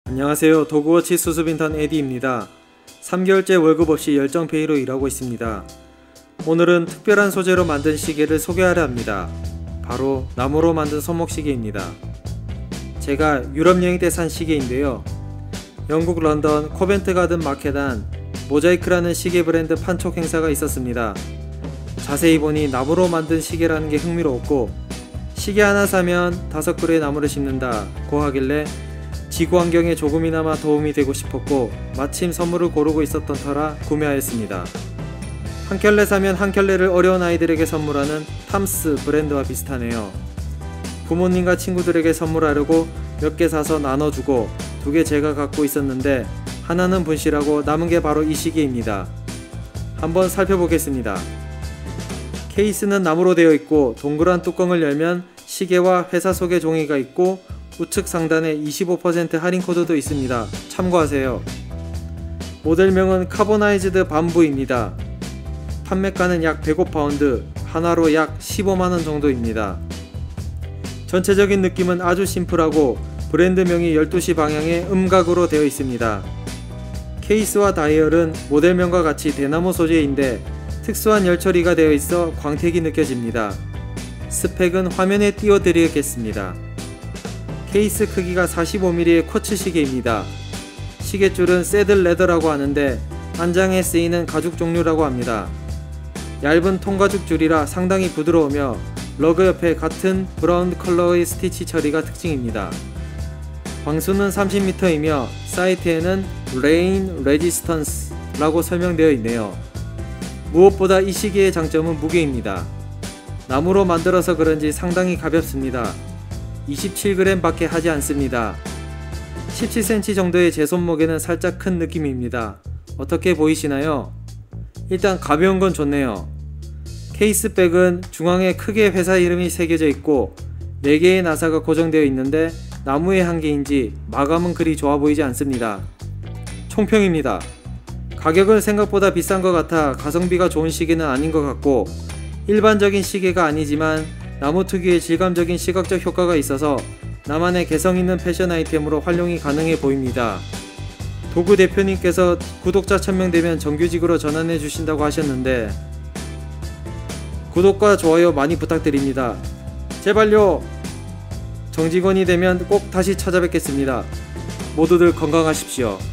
[도그스토리 #7] 영국 나무시계 모자이크 MOSAIQE 이제는 이런 시계까지 리뷰??
하다하다 도그워치가 이제는 나무로 만든 시계를 소개합니다.
저희 인턴직원이 영국에서 구입한 MOSAIQE 시계입니다.
원목소재 관련업계나 가구업계 종사자 분들께 추천드립니다^^.